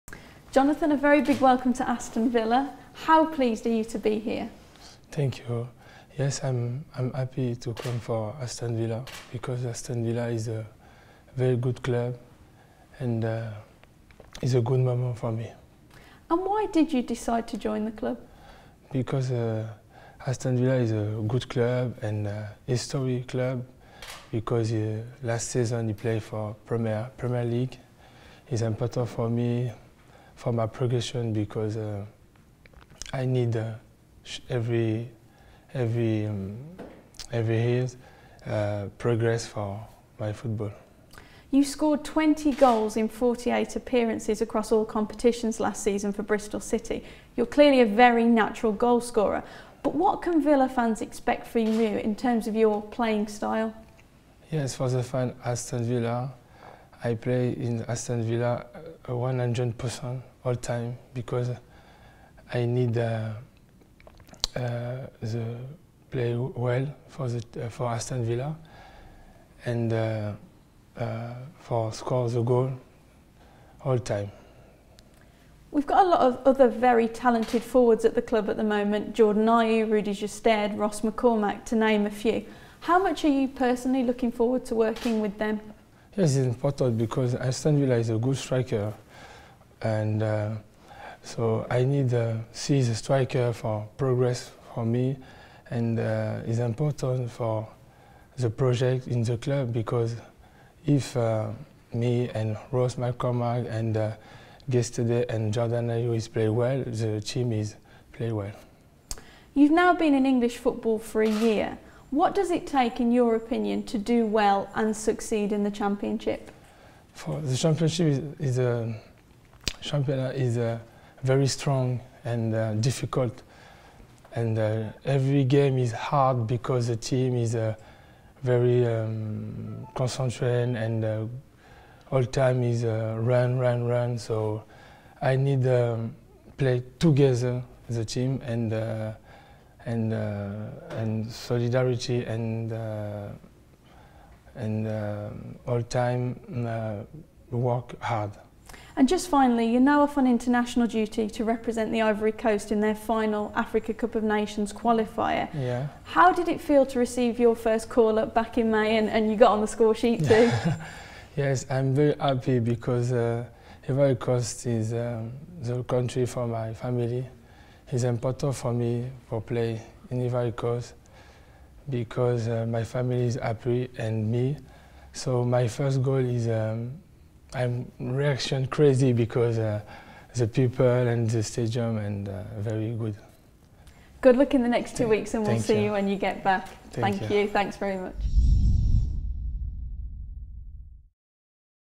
Jonathan Kodjia first interview
In case you missed it, we're now giving you access to watch the full first interview with AVTV after Jonathan Kodjia's arrival at Aston Villa in the Summer.